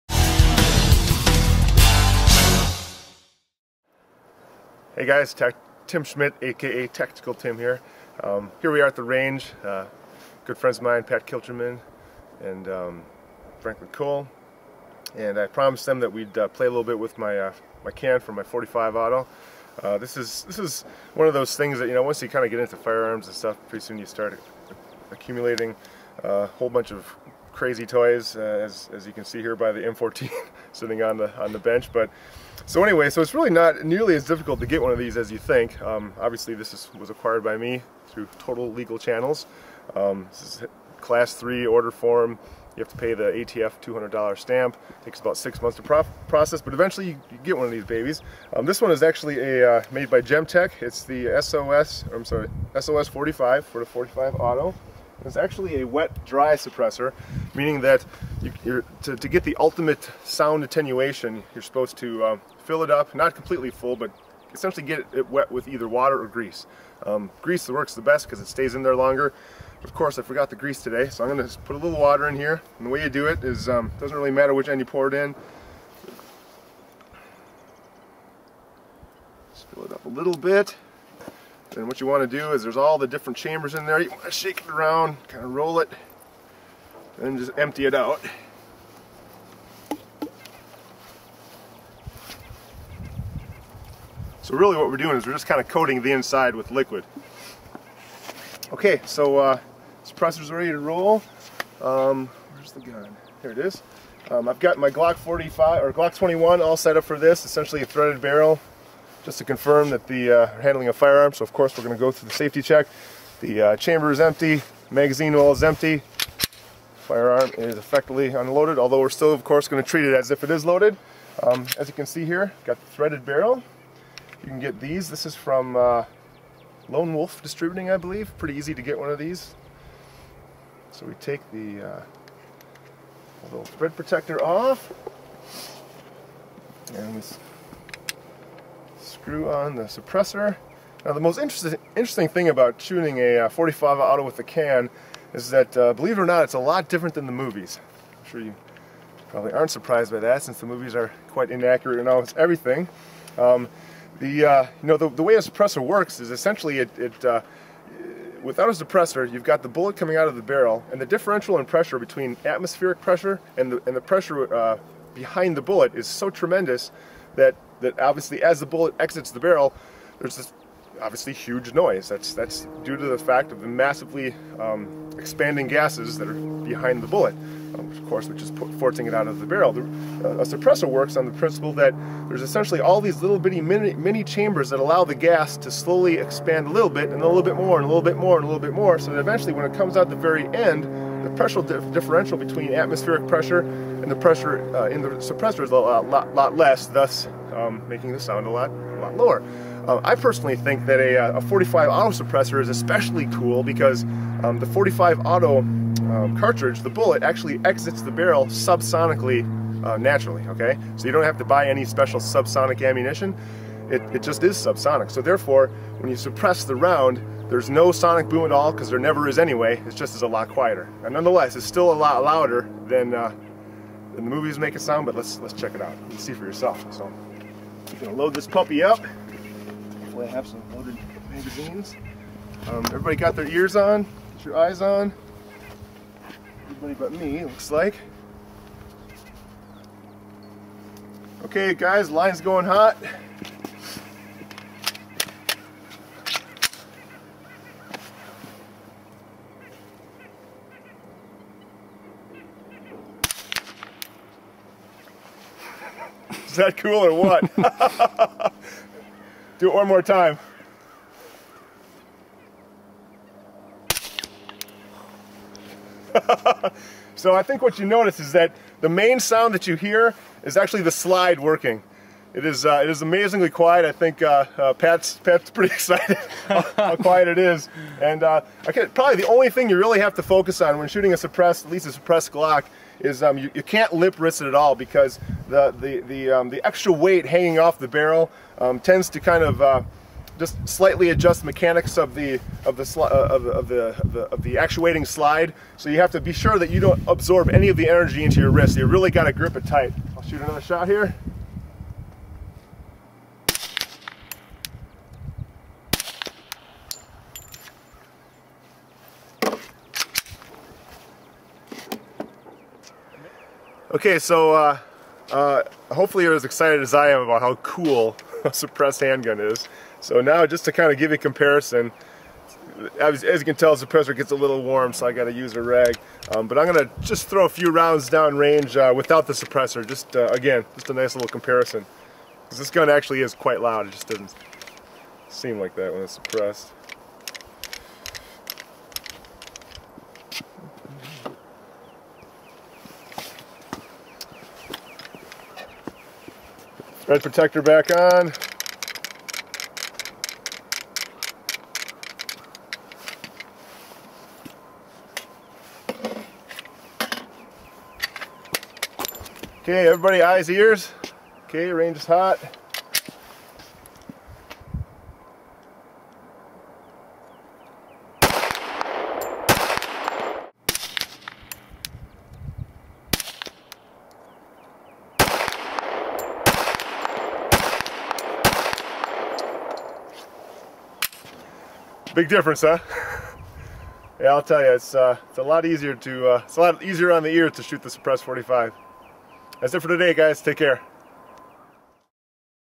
Silenced Glock 21
president "Tactical" Tim has some fun at the range and demonstrates the difference between shooting a Glock with and without a suppressor.  Learn the ins and outs of how a silencer actually works and see a live, side-by-side comparison of the silenced Glock in action.